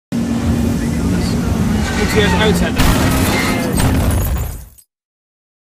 iPhone 4S Action Movie FX - Car Crash [ ghost protocol ]
This video was made on an iPhone 4S using Action Movie FX.